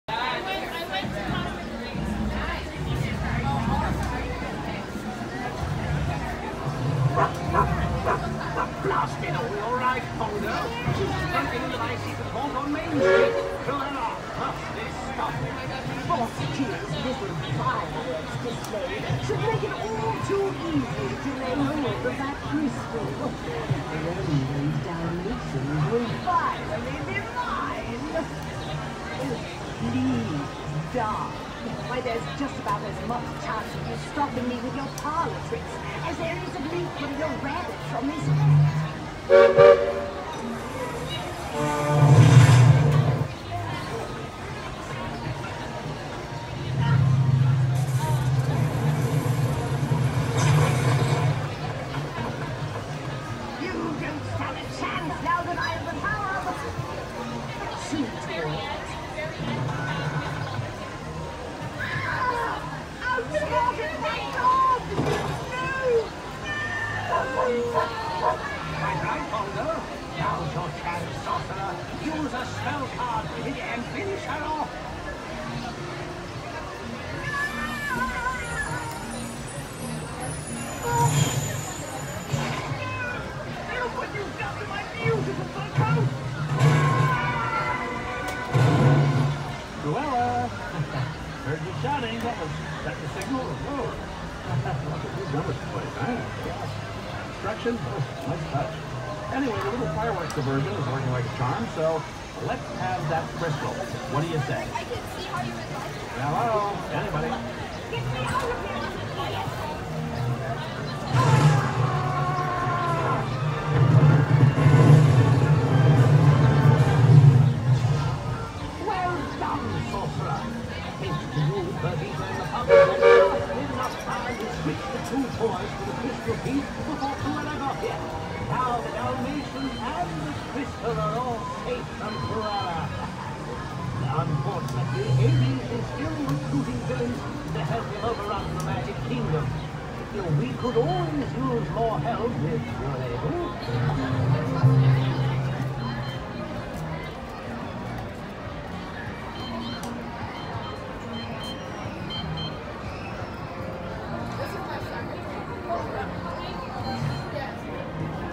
Sorcerers of the Magic Kingdom - Cruella Deville defeated - 4k
This is a video of the interactive scavenger hunt at the Magic Kingdom called Sorcerer's of the Magic Kingdom!  Enroll at the Fire station on Main Street, U.S.A. to join Merlin and become a Sorcerer to save the Magic Kingdom from Hades and all of the villains he's recruiting!  You'll find different portals throughout Main Street, U.S.A., Adventureland, Fronteirland, and Fantasyland along with different villains trying to find and steal a crystal to give to Hades.  This crystal holds the power of the Magic Kingdom so you must defeat each villains to restore the crystal back to Merlin!  Use different cards (you get a pack of 5 FREE cards when you visit the fire station) to defeat the villains!  This video is when Cruella de Vil is defeated!  We hope you enjoy the video and check out the other villains as well! This video was filmed in 4k resolution!